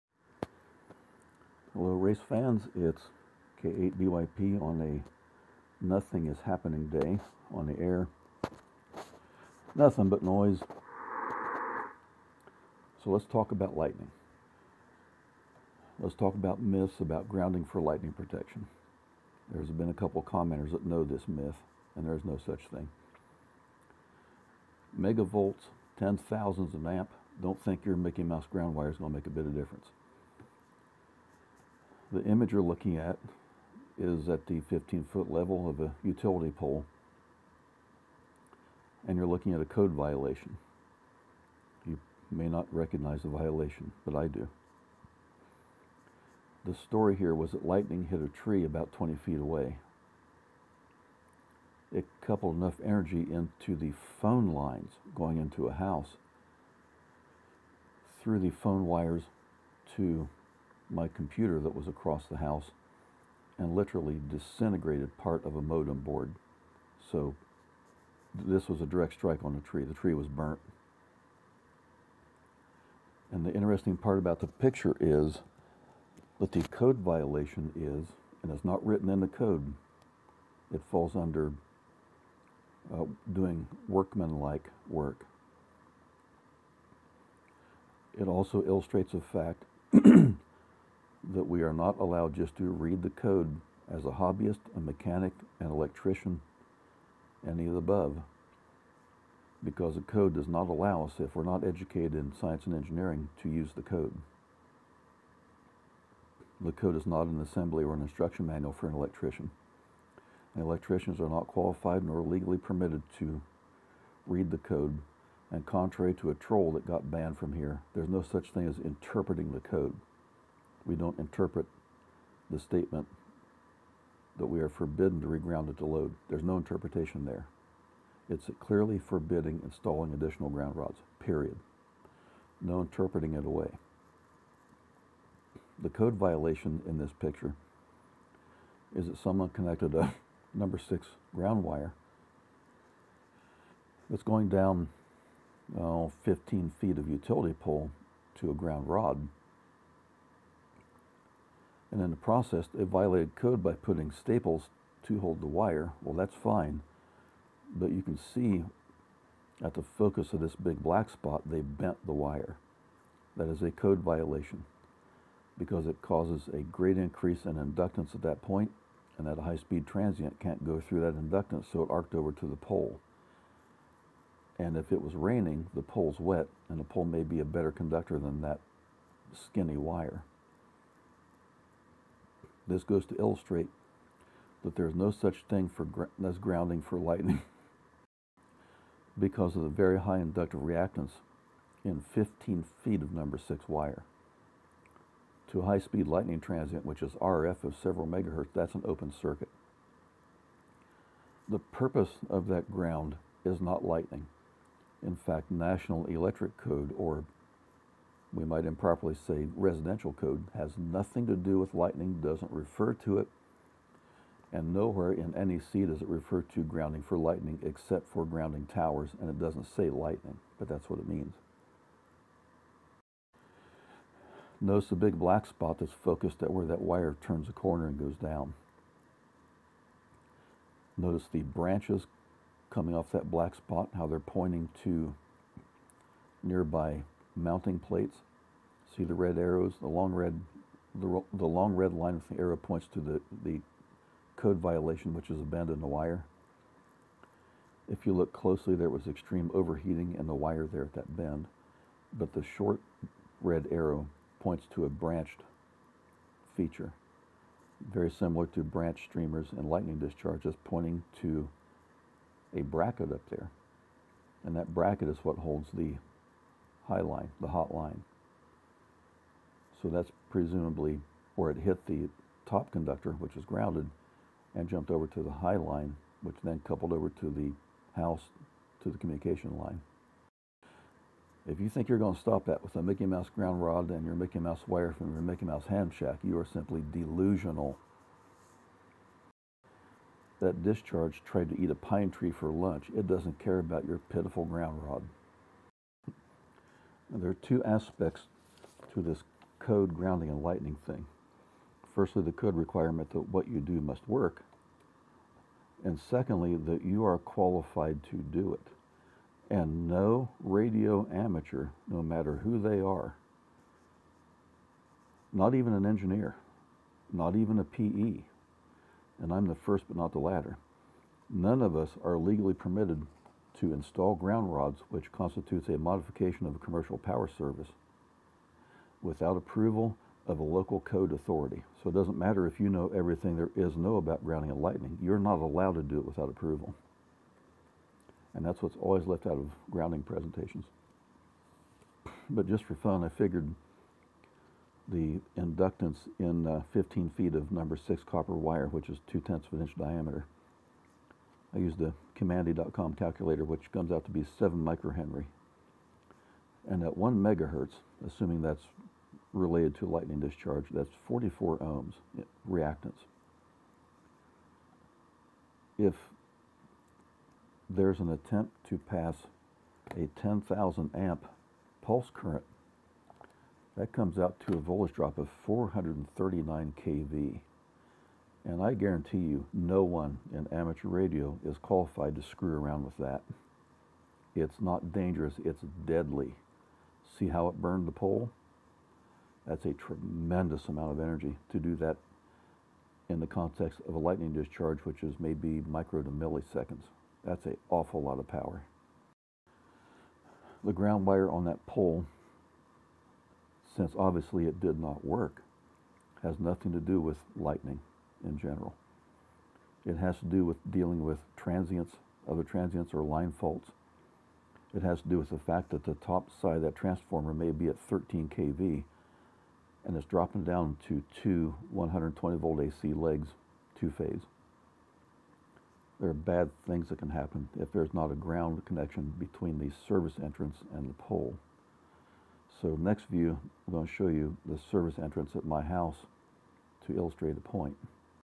Utility Pole Lightning Strike (NEC Violation)
Proof of why Code forbids bending ground wires. Since its not stated directly, poorly educated Electricians may not know this, and the electrical physics is WAY beyond them.

I didnt include the topic of my service drop as the video ran a bit long.

*THINK ABOUT THIS. what path did the lightning current take to get to the ground wire?* Hit the high lines OR from the TREE?